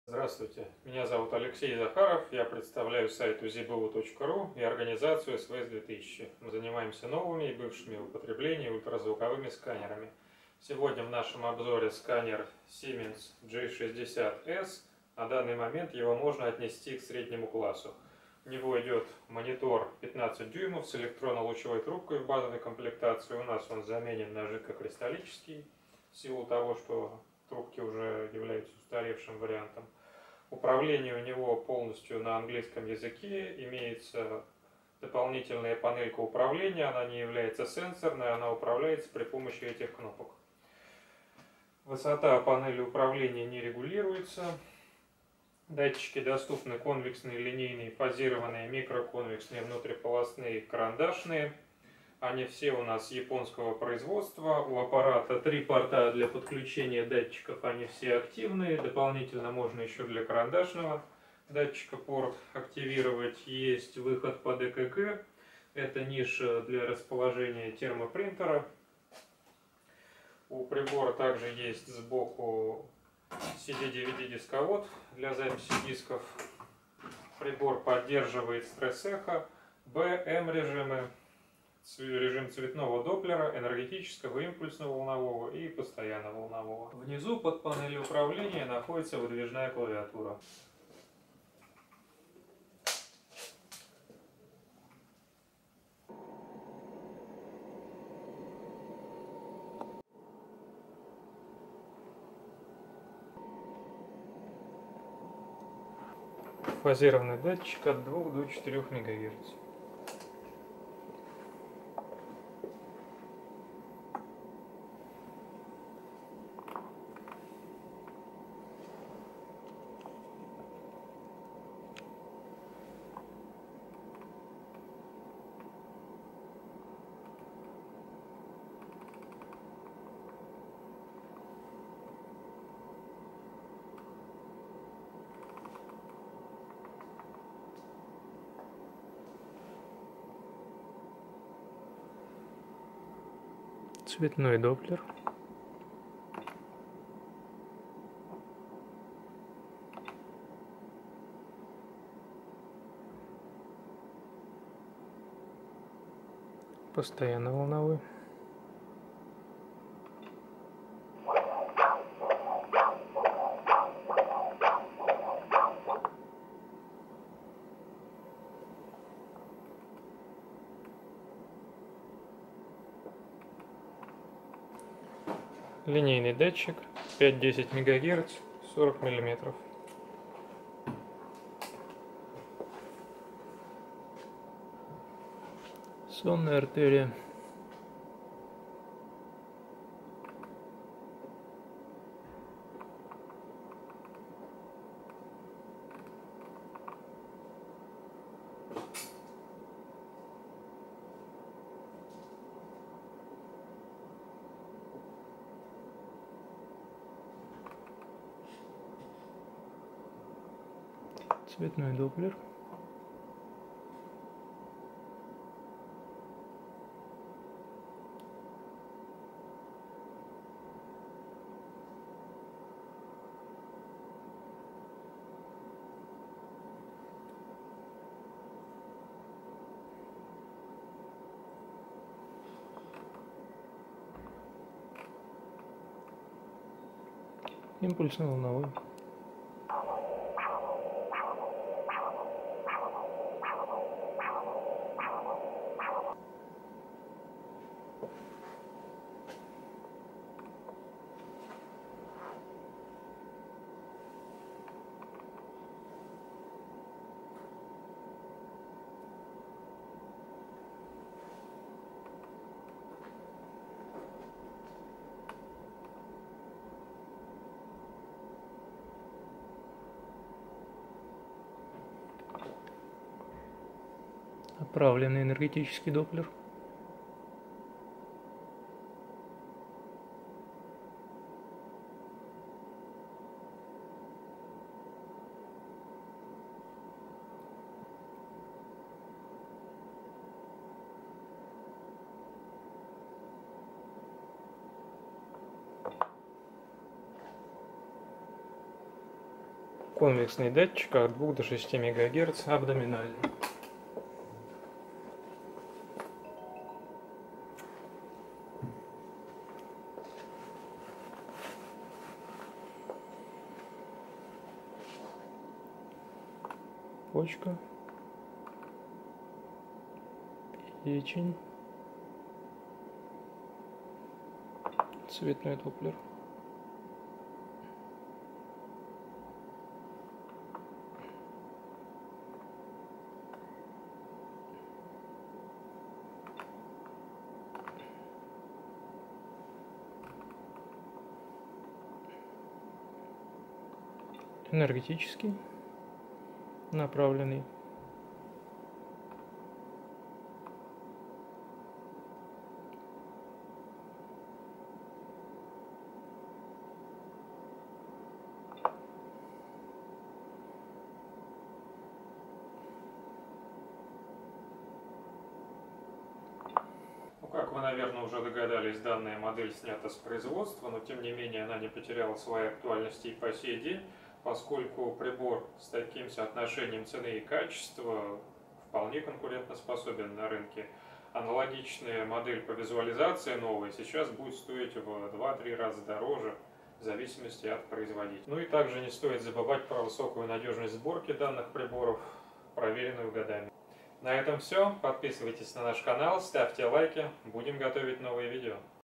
Siemens G60 обзор ультразвукового сканера
Купить данный узи аппарат и ознакомиться с ценами можно пройдя по ссылке Обратите внимание на настройку качества видео YouTube, она должна быть 1080p.